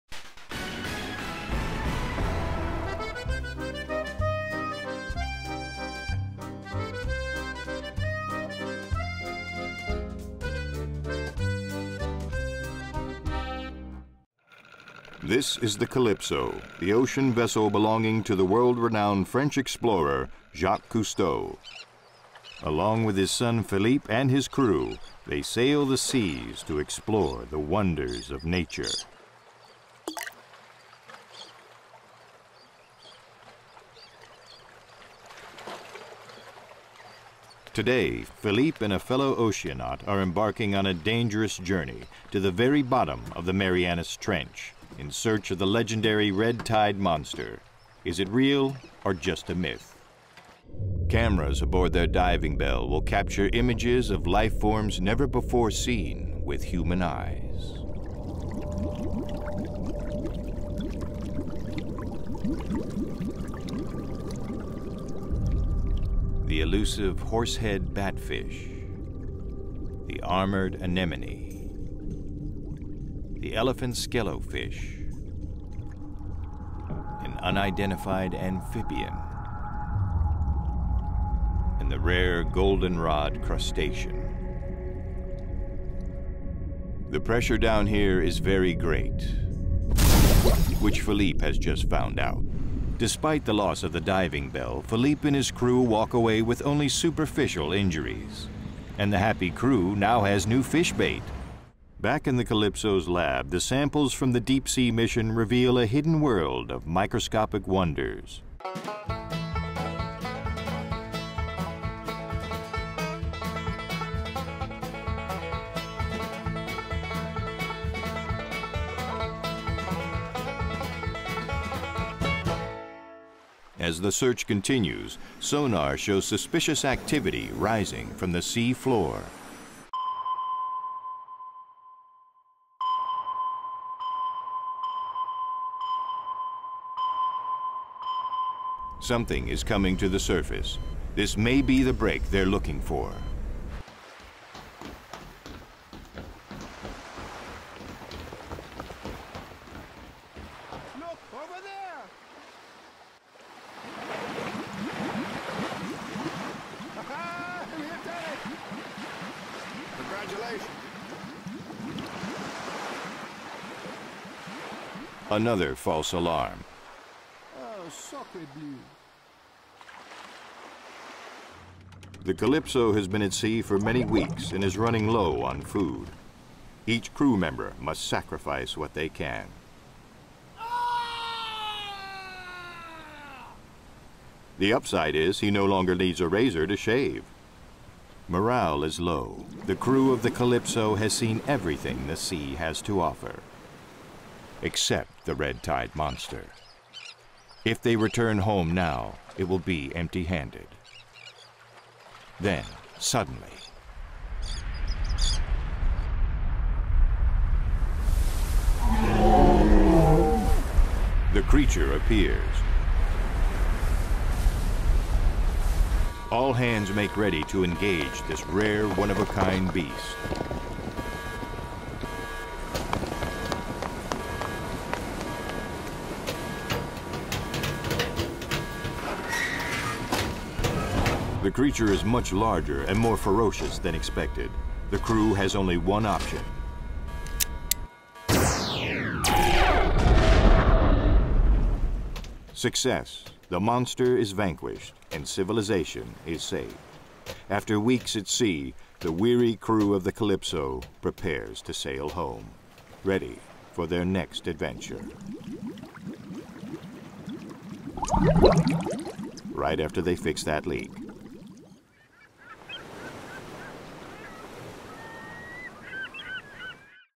The Waterlogged World Of Jacques Cousteau -- Super 8
Jacques Cousteau was a prominent figure when I was a lad & therefore ripe for parody in one of the many super 8 films I made with my younger brother & schoolyard friends. This film "The Waterlogged World of Jacques Cousteau" was made for a school project & our teacher--seen briefly before the titles-- was a man of good humor who generously indulged my creative whims. Since I'm directing, you won't SEE me in this film, but you will hear my narration reconstructed from a now long-lost cassette tape, and yes, we managed to slip in a Star Wars reference.😊 Fun Fact: We filmed this aboard the old USS Requin submarine, then docked in Tampa, now part of the Carnegie Science Center in Pittsburgh!